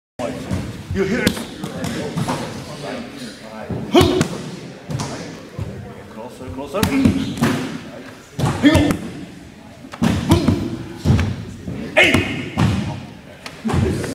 Sensei Yabe And Ryan
Sensei Yabe is teaching speed techniques. The objective is to beat your opponent to the punch.